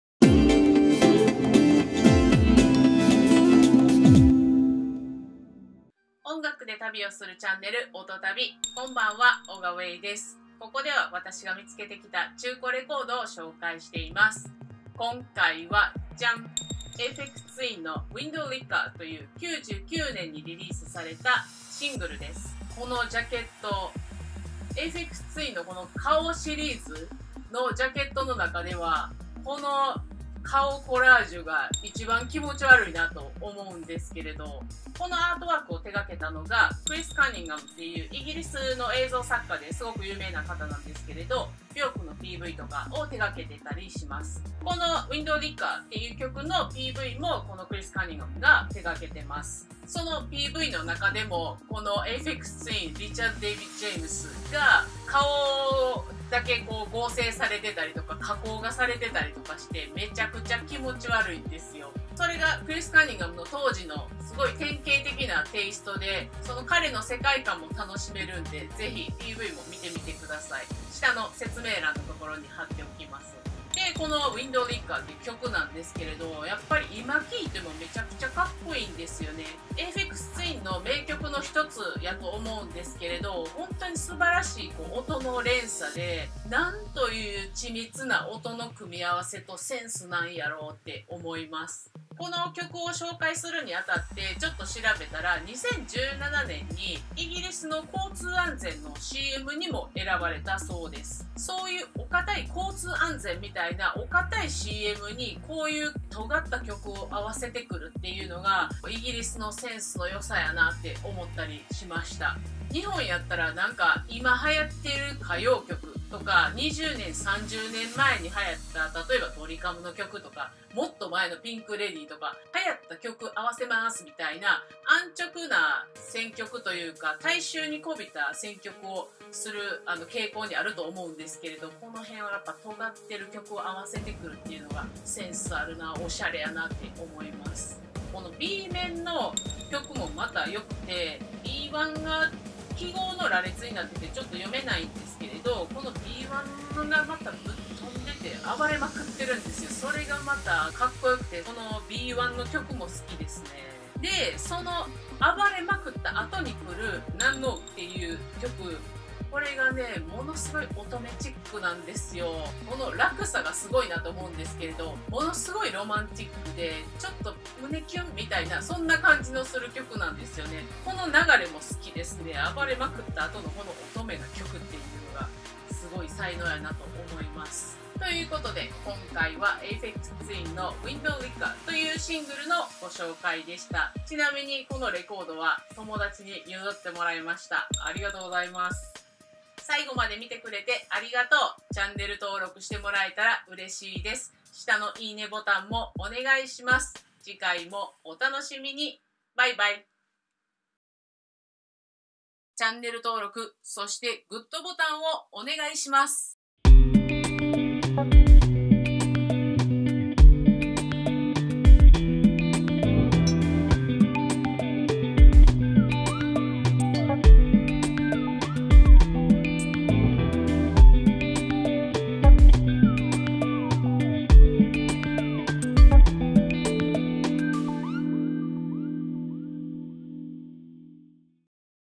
【Aphex Twin（エイフェックス・ツイン）】大名曲！Chris Cunninghamの顔コラージュジャケットにちなんで、キモイもん作ってみました。
【Aphex Twin（エイフェックス・ツイン）】大名曲！Chris Cunninghamの顔コラージュジャケット

このチャンネルでは、クラブミュージック、踊れる音楽を中心に
中古レコード屋で見つけてきたレコードを紹介しています。

「OTOTABI」オープニング&エンディング 作曲：Taro Kitamura